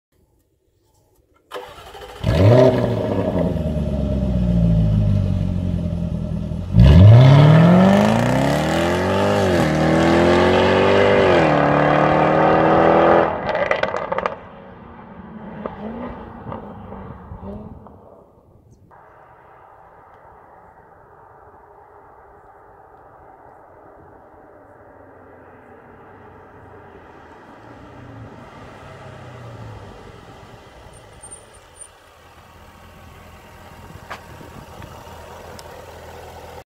Jaguar F-Type SVR - Zo klinken 575 trappelende paarden! - Driving-Dutchman
En zo klinken 575 trappelende Engelse paarden! Dit is de Jaguar F-type SVR!